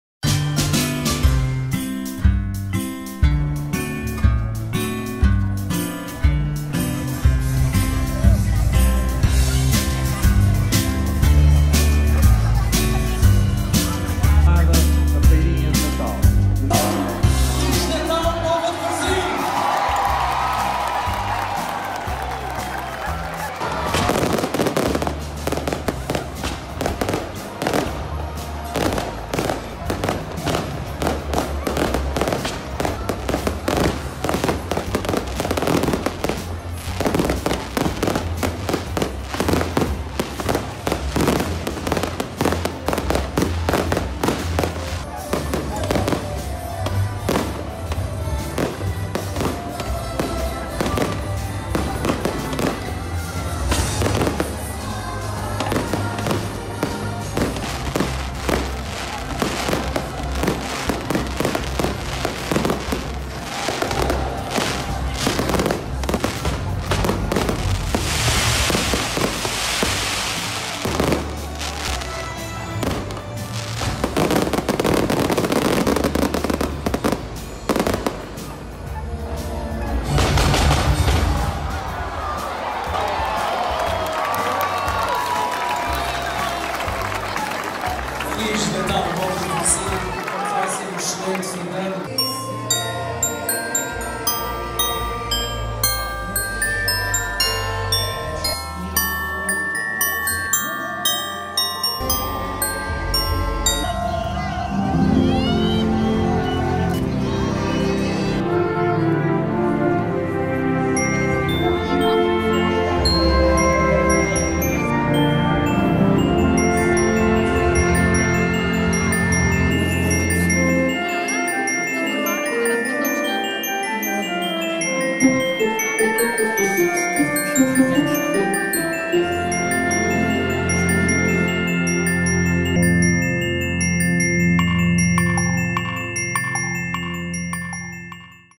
Iluminações de Natal 2022, Póvoa de Varzim
A 26 de novembro 2022, a Póvoa de Varzim inaugurou a iluminação de Natal com um espetáculo na praça do Almada.

Subscreva este canal gratuitamente para ter acesso a mais vídeos
@pmo TV